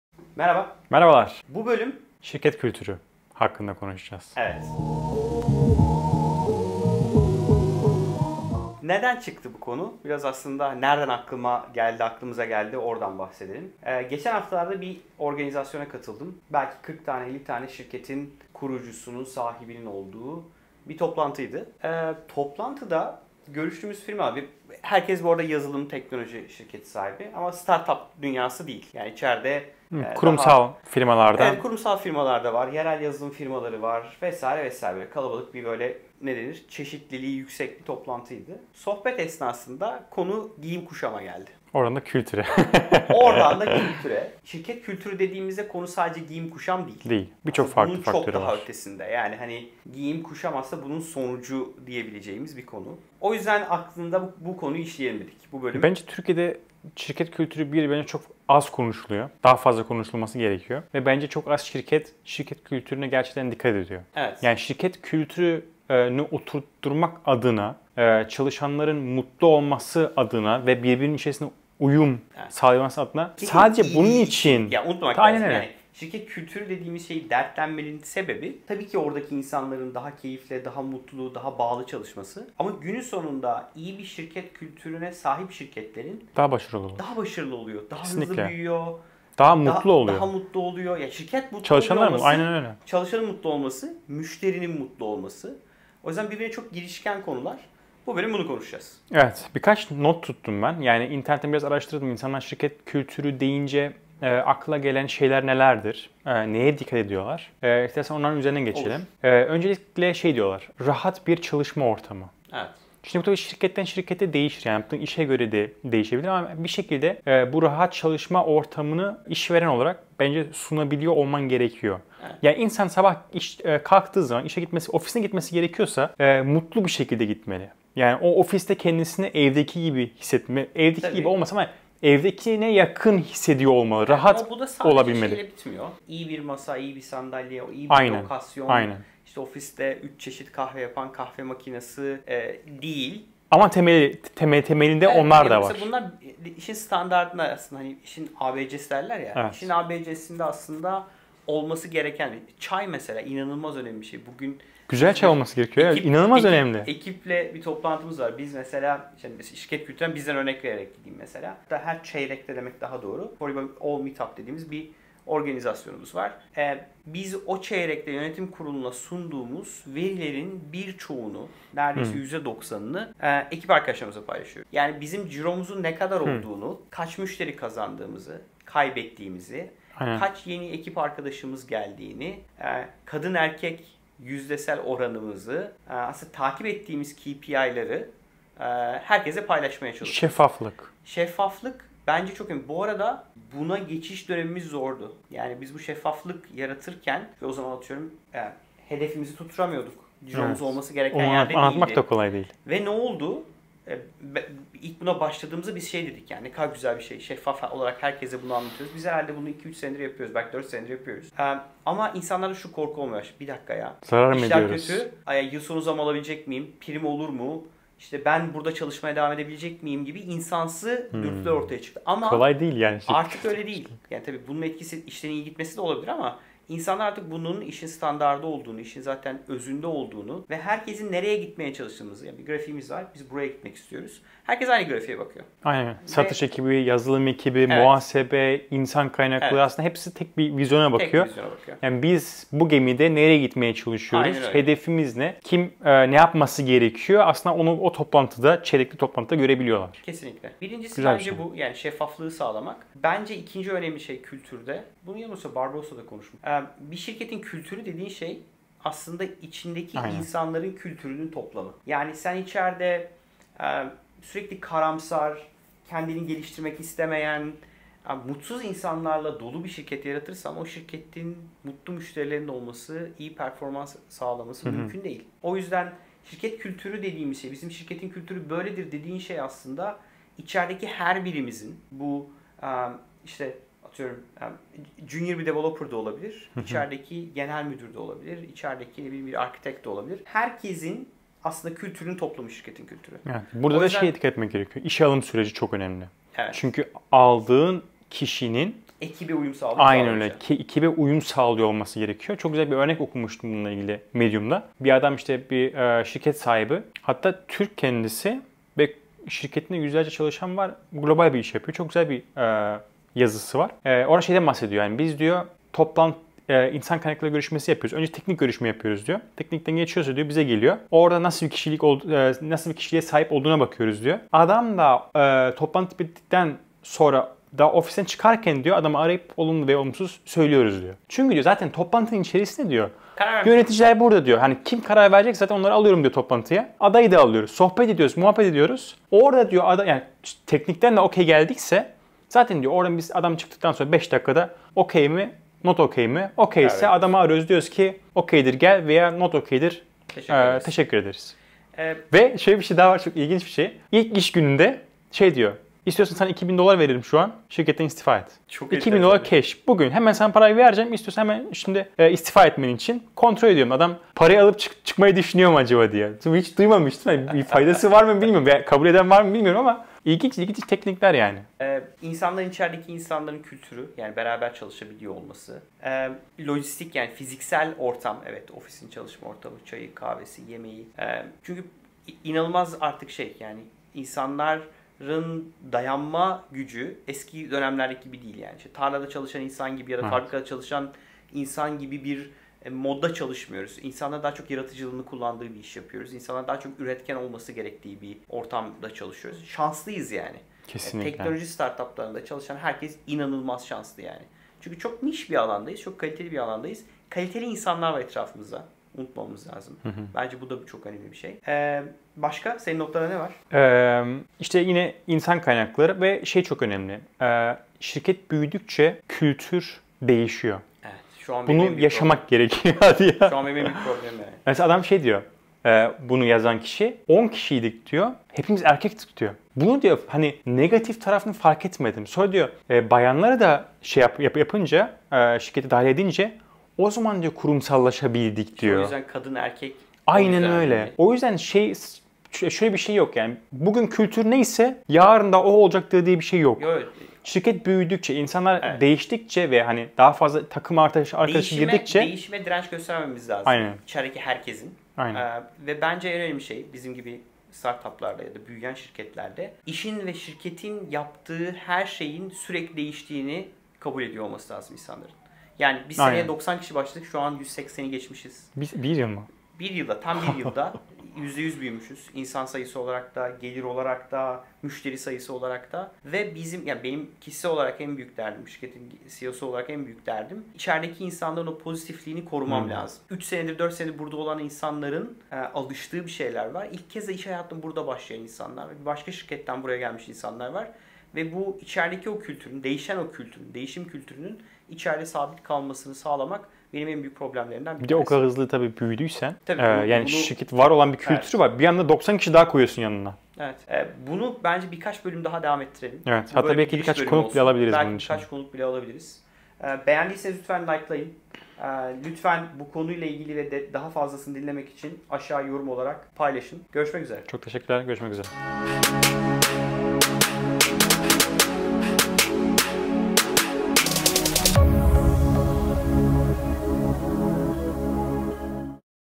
#156 Girişimlerde Şirket Kültürü Nasıl Oluşturulur
Bu bölümde şirket kültürü hakkında konuştuk. Özellikle girişimler bu konuda neler yapıyor ve nelere dikkat ediyoru merak ediyorsanız bu bölümü izlemenizi tavsiye ederiz.